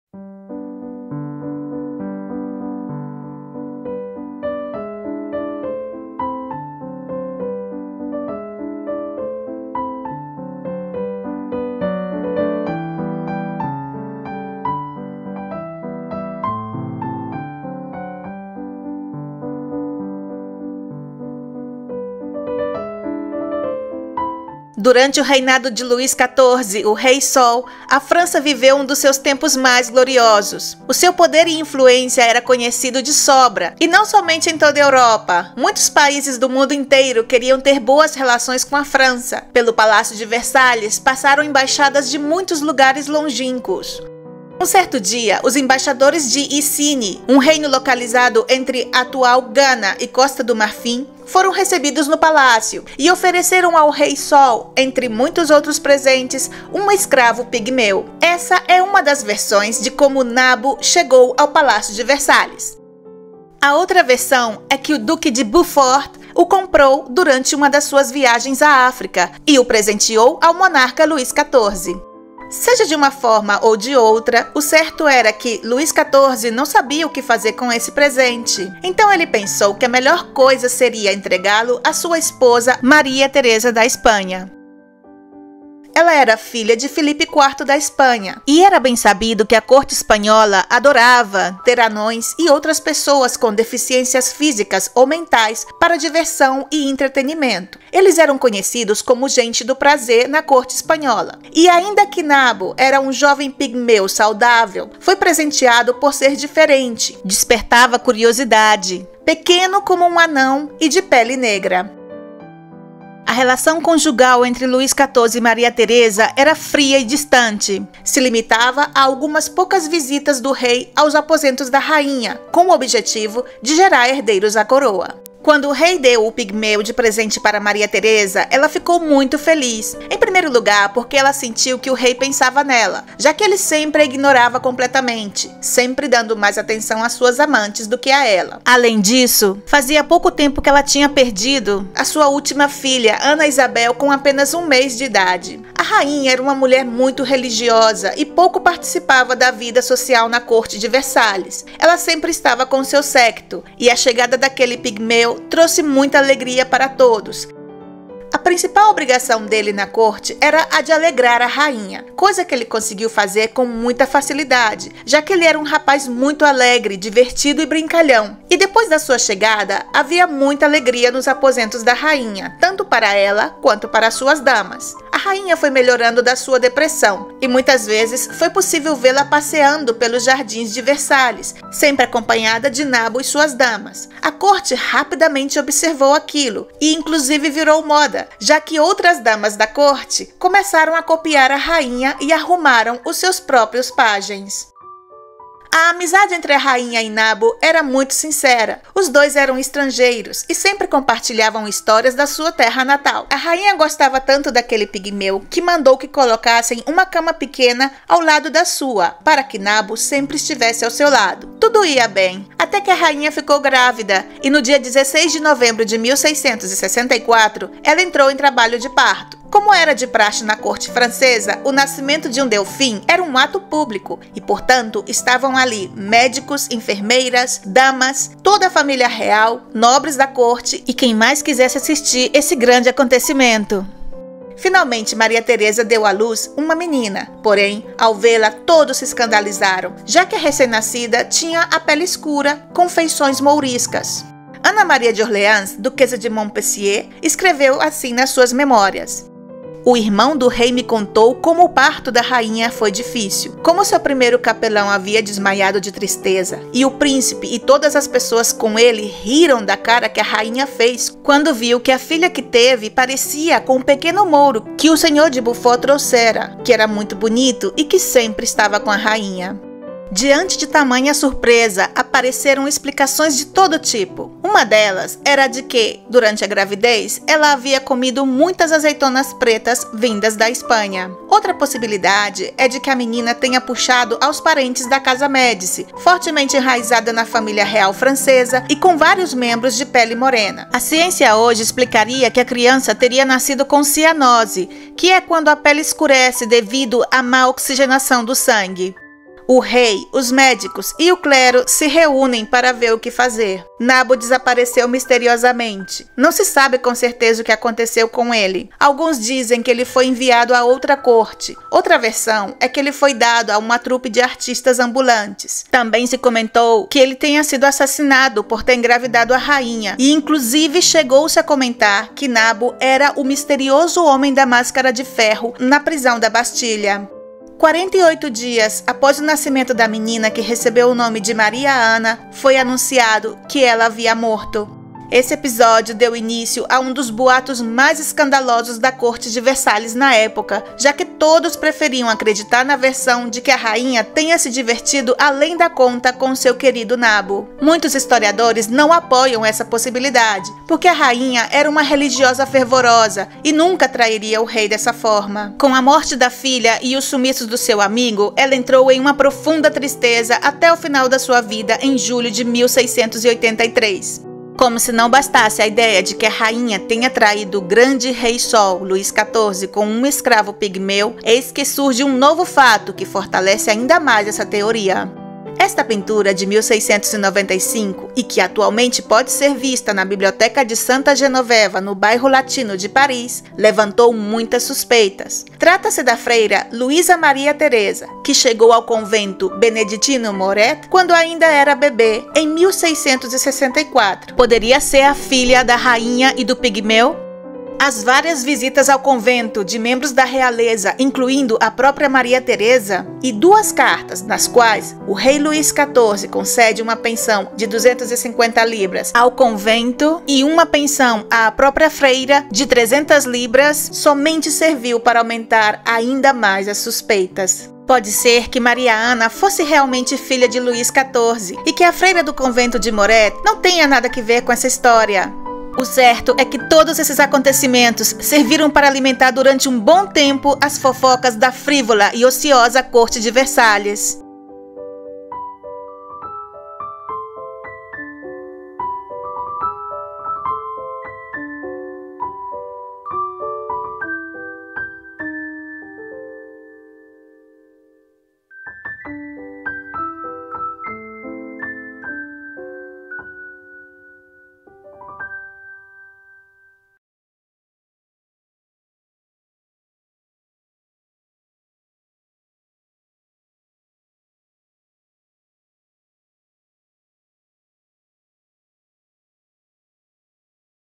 Nabo. O Escravo Pigmeu da corte de LUIS XIV. Conquistou o coração da Rainha.
A principal obrigação de Nabo na corte, era a de alegrar a rainha, coisa que ele conseguiu fazer com certa facilidade já que ele era um rapaz muito alegre, divertido e brincalhão.  Depois da sua chegada havia muita alegria nos aposentos da rainha, tanto para ela como para as suas damas de companhia.

Tudo ia bem Até a rainha ficou grávida e no dia 16 de novembro de 1664 ela entrou em trabalho de parto, deu à luz uma menina,  porém ao ve-la  todos se escandalizaram já que a que recem-nascida era pele escura, com feições mouriscas.

Muito obrigada pelo seu apoio!💟